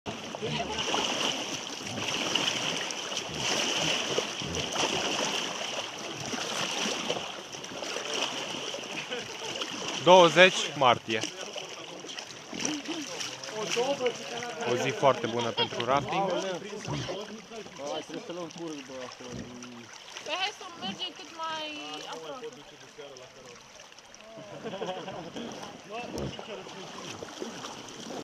NERA-thlon 2010 - Rafting ape linistite3
NERA-thlon 2010 - Incearca-ti puterile: coboara dealuri in mare viteza cu bicicleta, pedaleaza pe drumuri de munte, fa drumetii ziua si noaptea, fa rapel coborand intr-o prapastie de 15m, arunca-te pe o tiroliana lunga de 84m, fa rafting pe cele mai lungi chei din tara (22km). Fii activ si infrunta cu bine si celelalte surprize pregatite de noi.